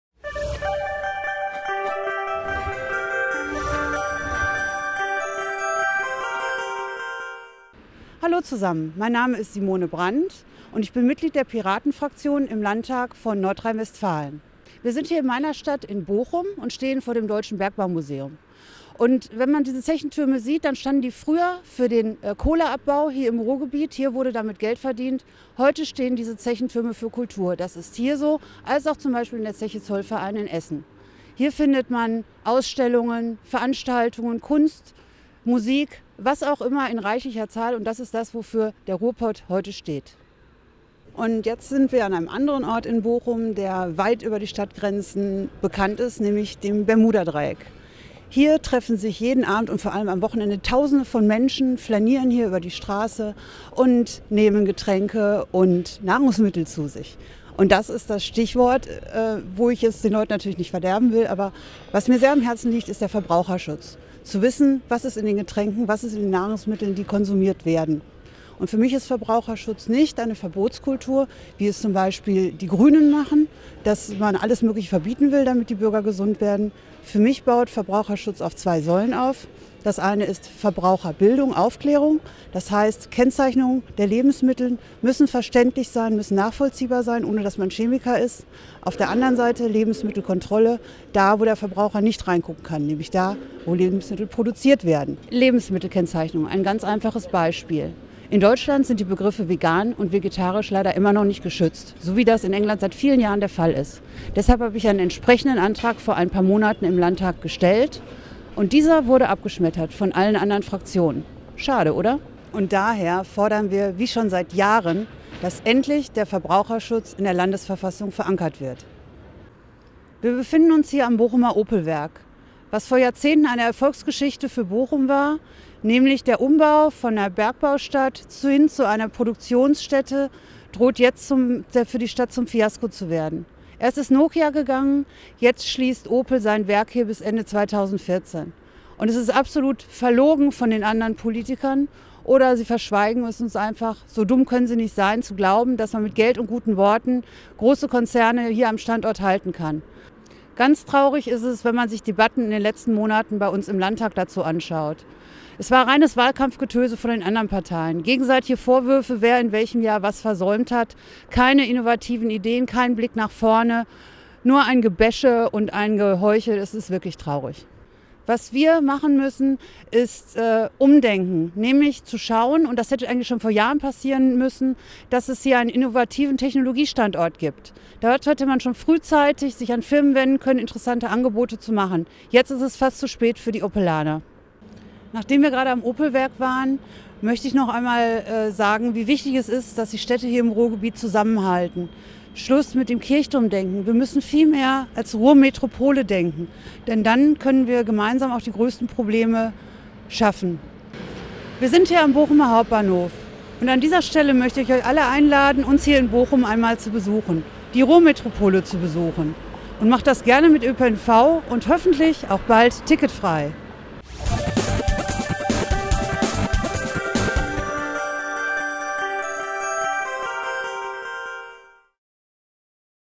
Simone Brand (@piratringwraith) stellt sich und ihre Heimatstadt Bochum vor
Simone Brand ist gebürtige Bochumerin und bekennende Ruhrpottlerin. Im Video zeigt sie drei Orte, die symbolisch für die Stadt und für die ganze Region sind.